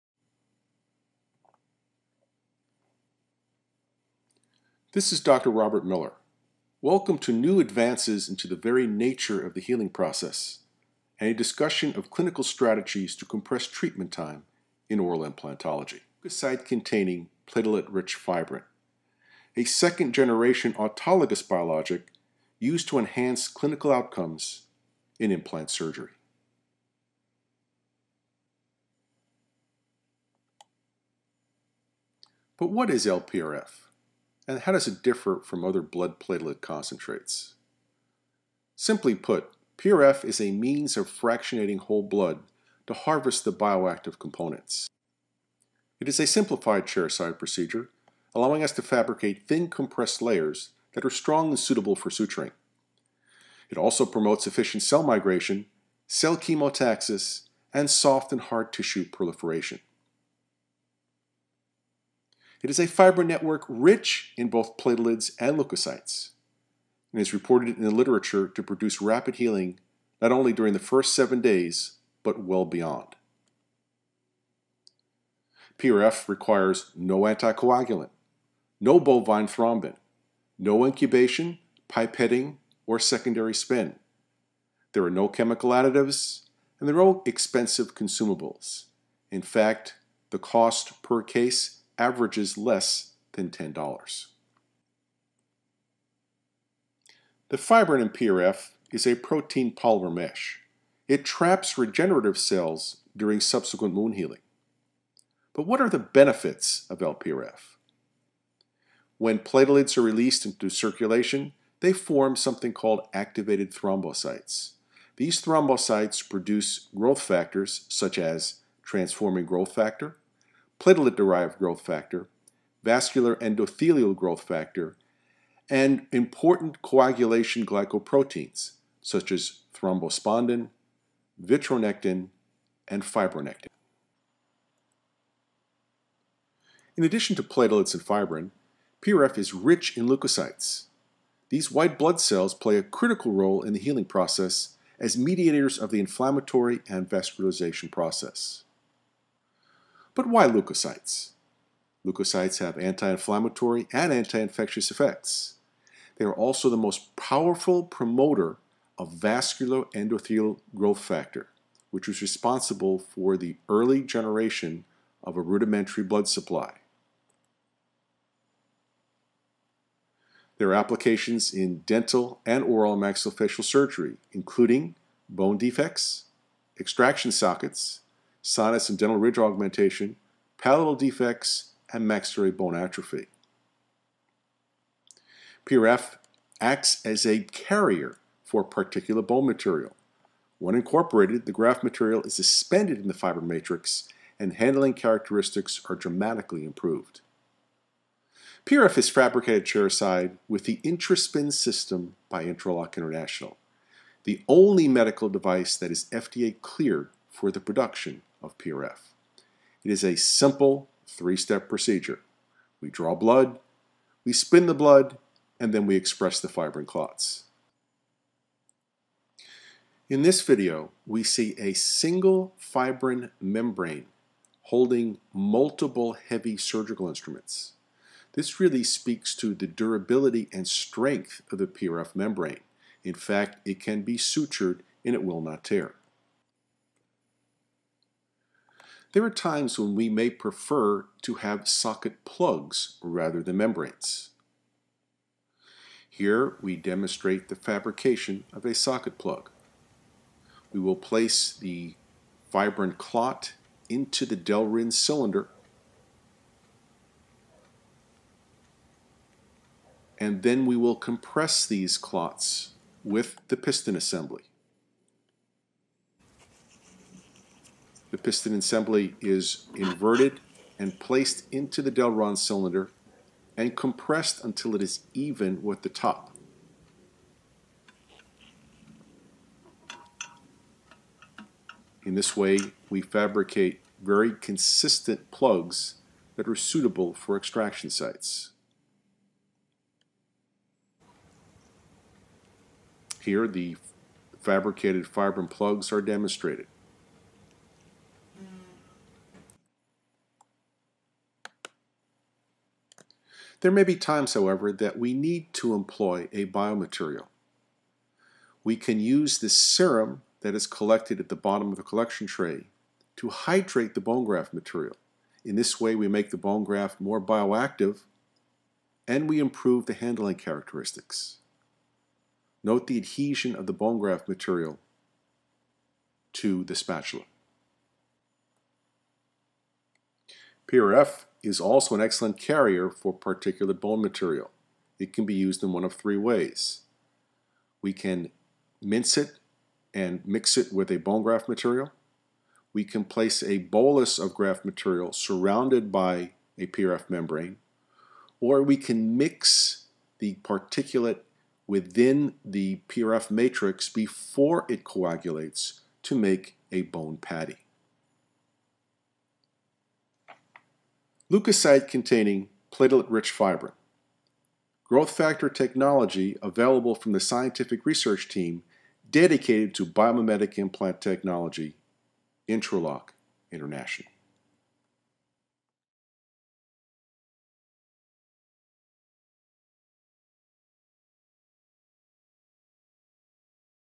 Why L-PRF (Leukocyte-Platelet Rich Fibrin)?
L-PRF™ is a 3-D autogenous combination of Platelet Rich Fibrin derived from the patient’s blood. A simplified chairside procedure results in the production of a thin, compressed layer of platelet rich fibrin that is strong, pliable and suitable for suturing. This natural fibrin network is rich in platelets, growth factors and cytokines that are derived from the blood platelets and leukocytes.

The presence of these proteins have been reported to produce rapid healing, especially during the critical first seven days after placement. This network promotes more efficient cell migration and proliferation without chemical or bovine thrombin additives.

Clinically, Leukocyte-Platelet Rich Fibrin displays excellent working properties. This biomaterial is resilient, strong and pliable, making it easy to manipulate. It can be cut to size, and is supple enough to adapt to many anatomical areas. It is adhesive in nature and very receptive to suturing. In addition, there is ample working time since L-PRF™ is stable at room temperature for several hours.